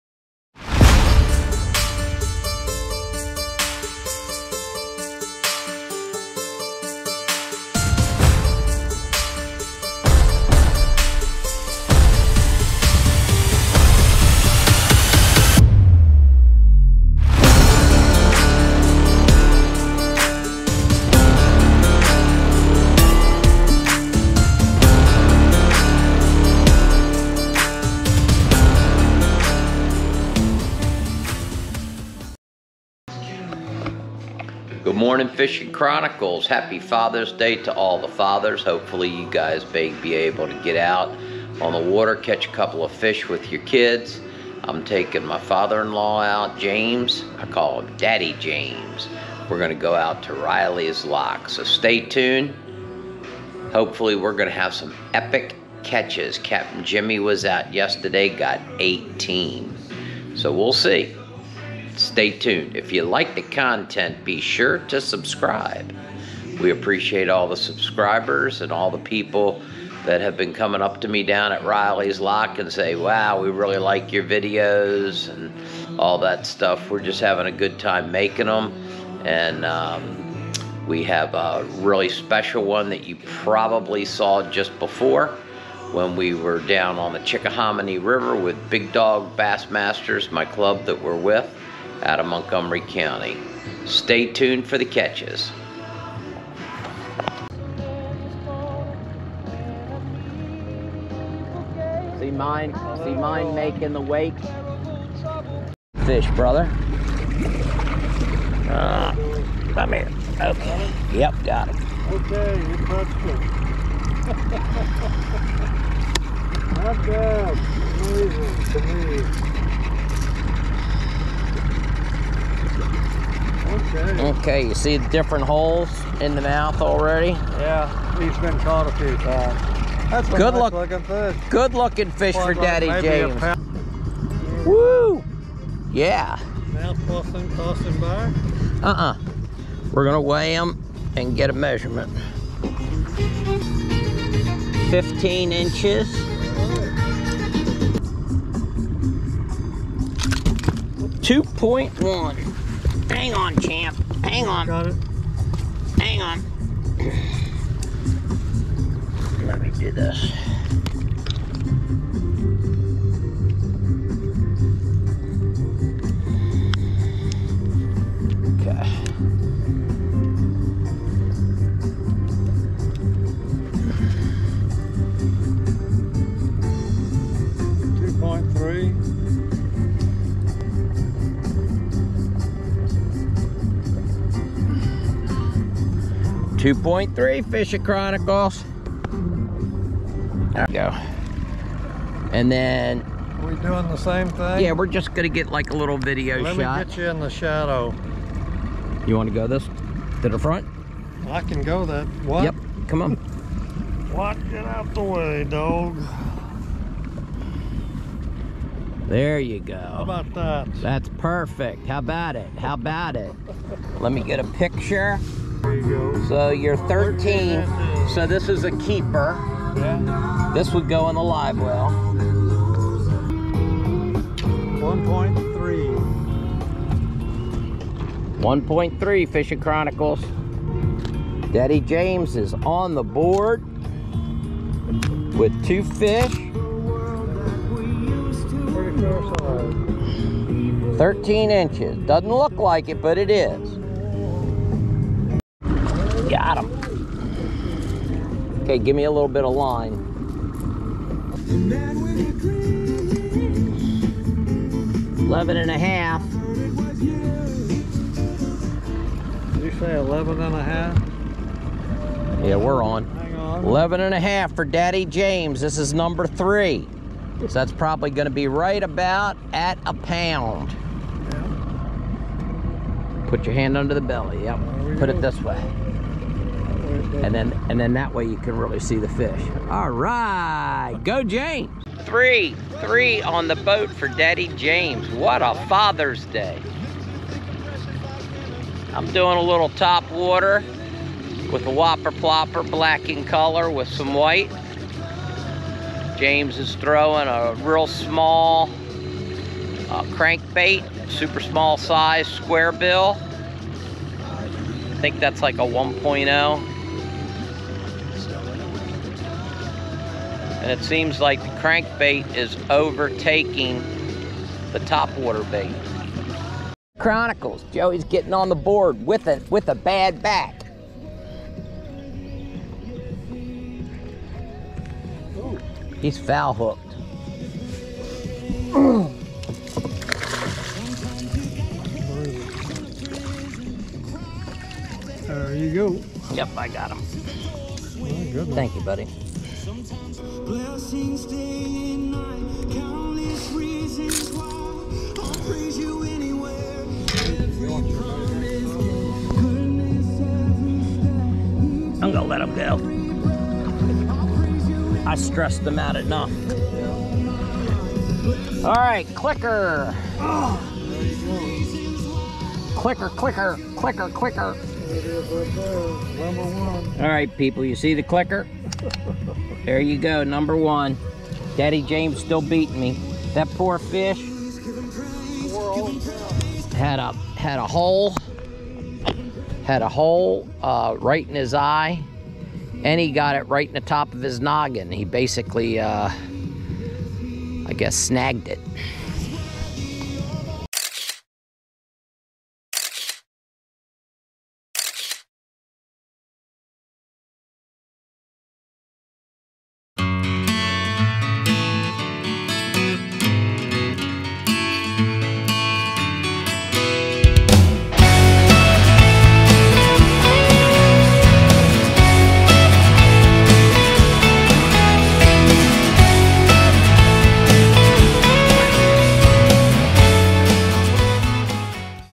Father's Day Fishing with Daddy James | Riley's Lock | MD
Hey, Fishin' Chronicles, Craig and I went fishing at Riley's Lock.
Happy Father's Day to all the fathers out there.